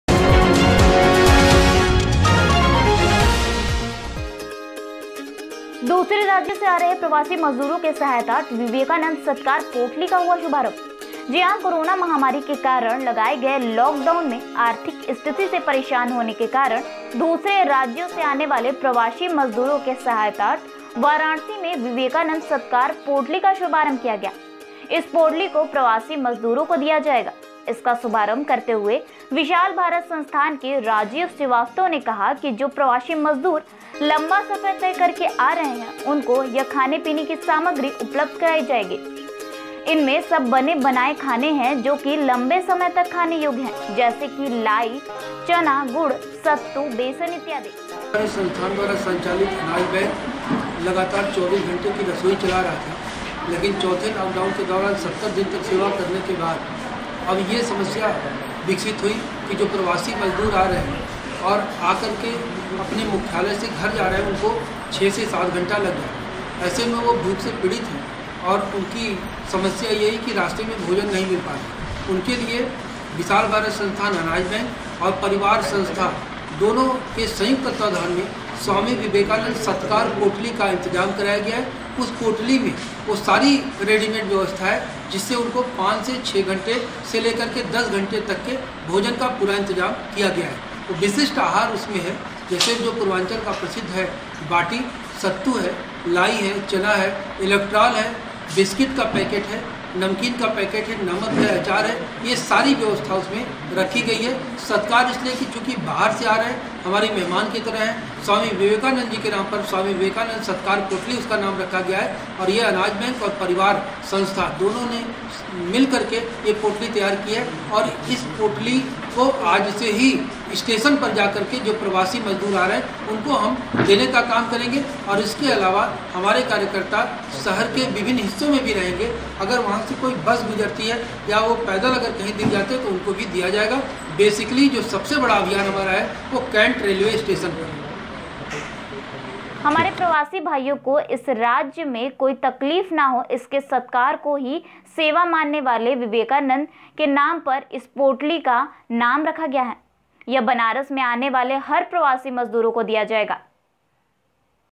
दूसरे राज्यों से आ रहे प्रवासी मजदूरों के सहायतार्थ विवेकानंद सत्कार पोटली का हुआ शुभारंभ
वाराणसी - करोना महामारी के कारण लगाए गए लाख डाउन में आर्थिक स्थिति से परेशान होने के कारण दूसरे राज्यों से आने वाले प्रवासी मजदूरों के सहायतार्थ वाराणसी में विवेकानंद सत्कार पोटली का शुभारंभ हुआ इस पोटली को प्रवासी मजदूरों को दिया जाएगा इसका शुभारंभ करते हुए विशाल भारत संस्थान के राजीव श्रीवास्तव ने कहा कि जो प्रवासी मजदूर लंबा सफर तय करके आ रहे हैं उनको यह खाने-पीने की सामग्री उपलब्ध करवाई जाएगी इसमें सब बना बनाया खाना है जो कि लंबे समय तक खाने योग्य है जैसे कि लाई चना गुड़ सत्तू बेसन इत्यादि हमारे प्रवासी भाइयों को इस राज्य में कोई तकलीफ ना हो इसलिए सत्कार को ही सेवा मानने वाले विवेकानंद जी के नाम पर इस पोटली का नाम रखा गया है यह बनारस में आने वाले हर प्रवासी मजदूर को दिया जाएगा